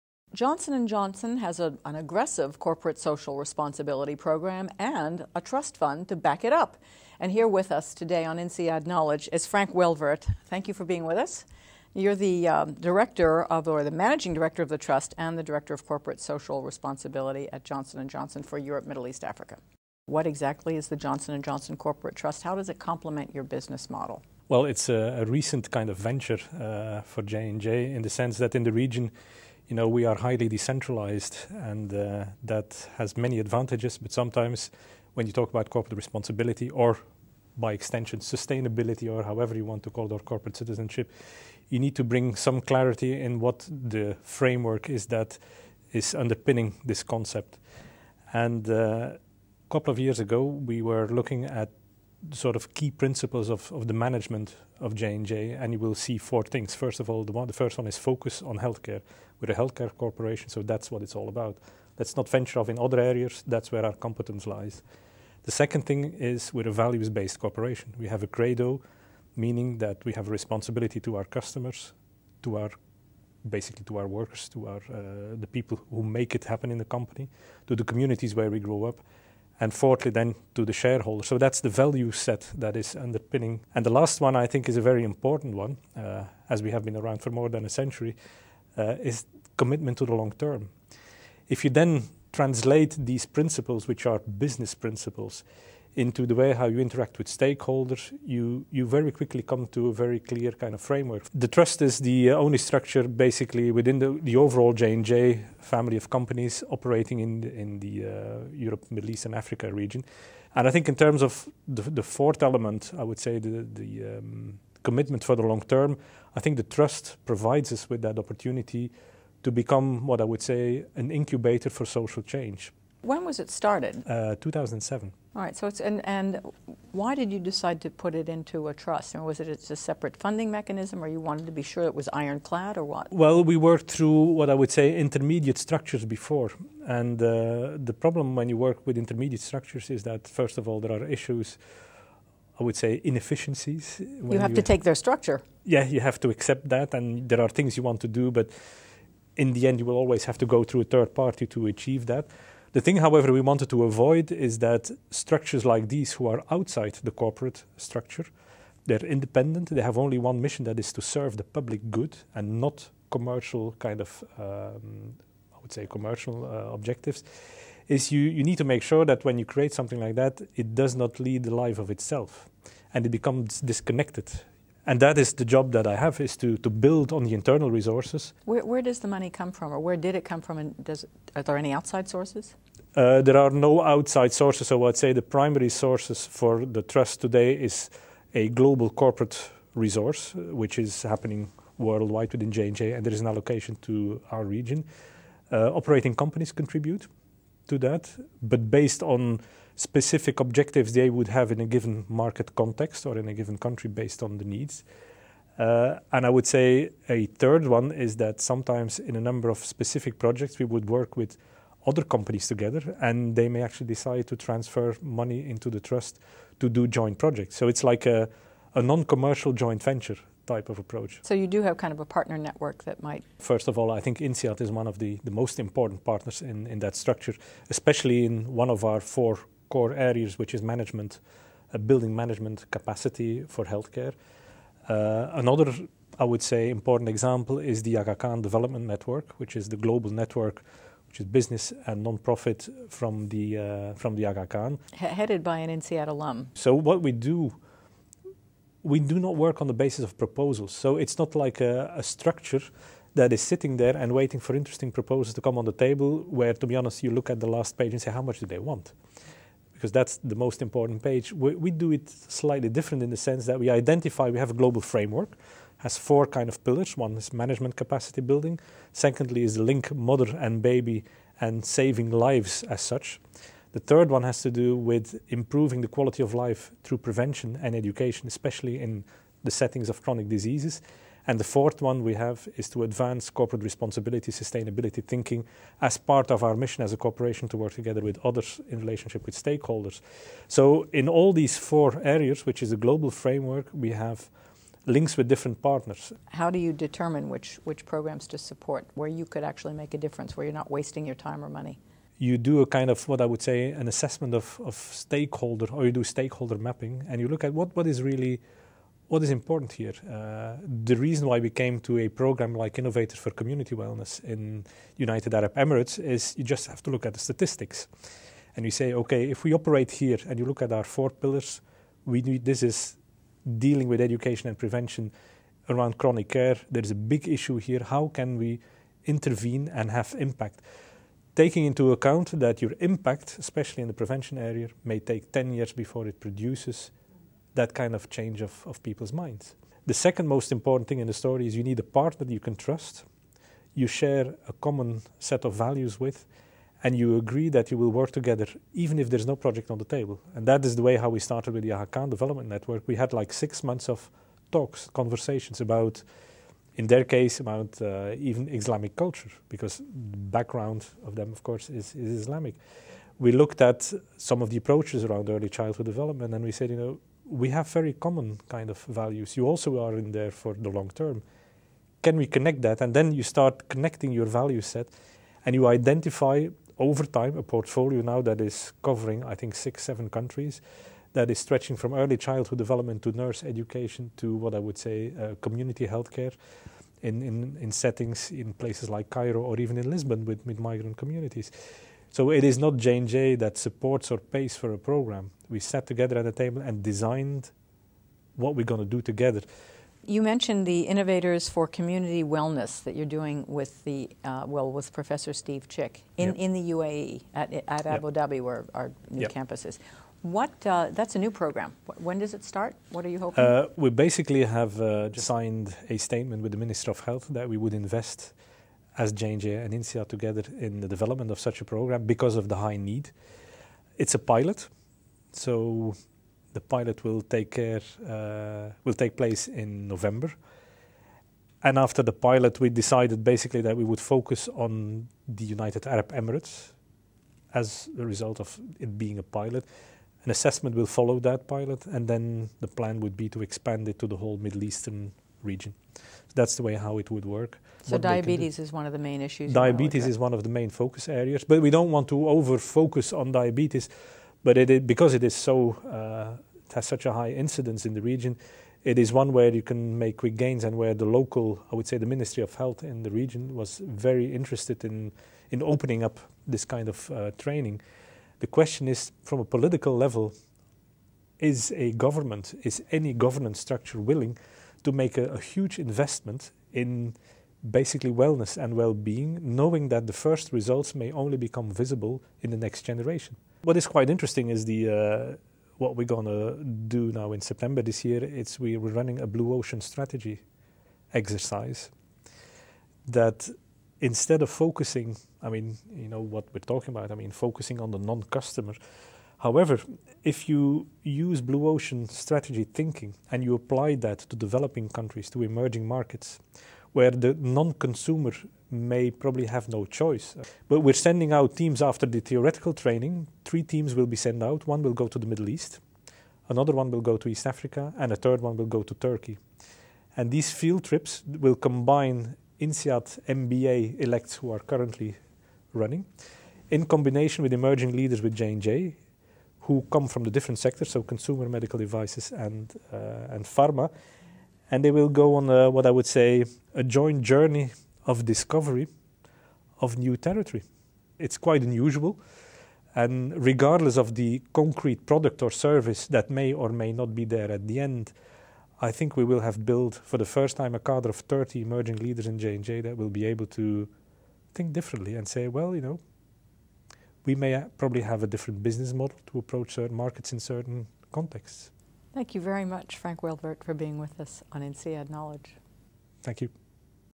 Frank Welvaert of Johnson & Johnson Trust on grassroots philanthropy
Frank Welvaert, managing director of the Johnson & Johnson Trust, talks about the group's mission to fund partnerships and grassroots philanthropy in the EMEA region.